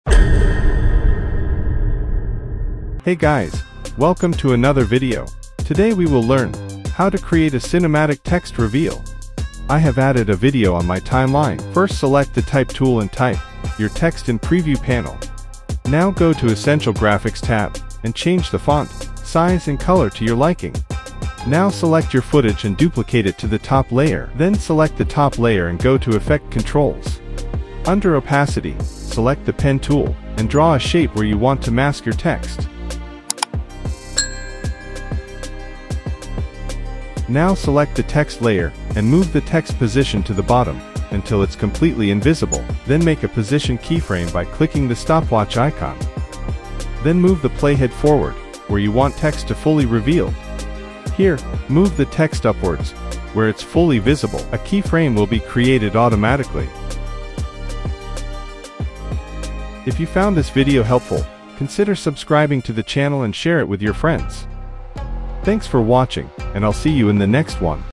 How to Create Cinematic Text Reveal in Adobe Premiere Pro
🎥 Learn How to Create Cinematic Text Reveal in Adobe Premiere Pro!

Welcome to our latest tutorial! In this video, we'll show you the easiest way to Create Cinematic Text Reveal in Adobe Premiere Pro.

🔹 What You'll Learn:

- How To Use Opacity and Masking
- How To Create Cinematic Text Reveal